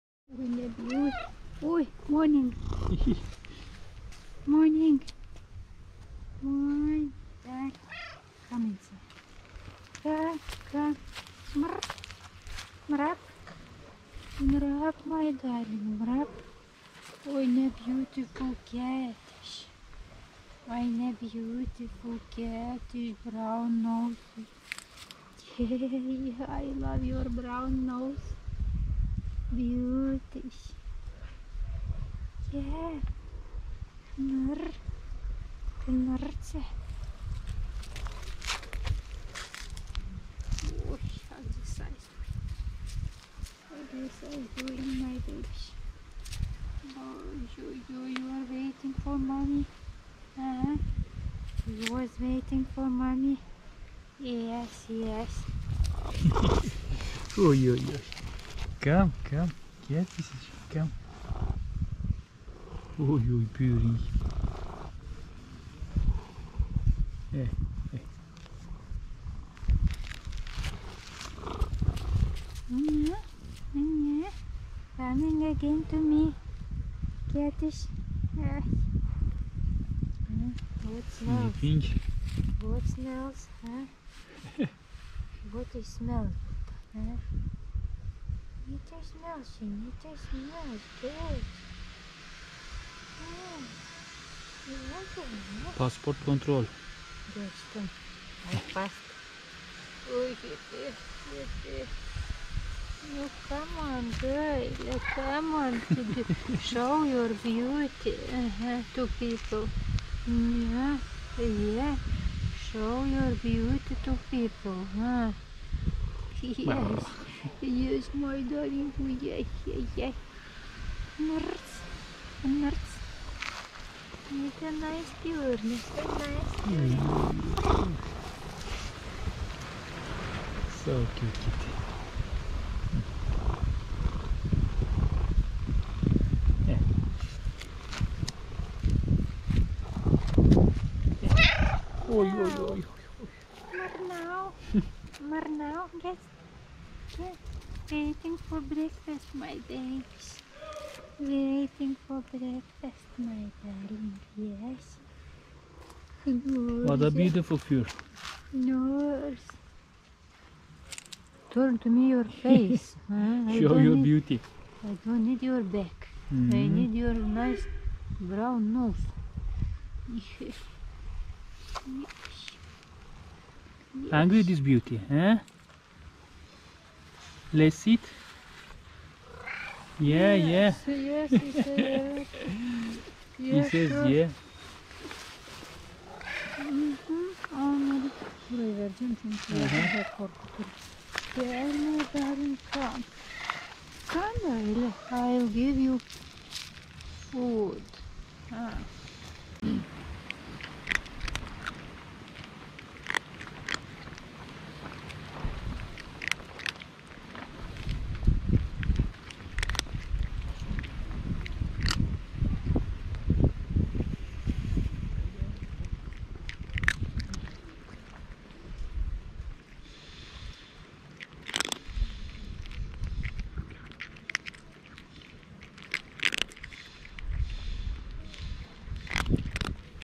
Beautiful cat asks me for food with her cutest meow
Beautiful cat asks me for food with her cutest meow because she is so hungry.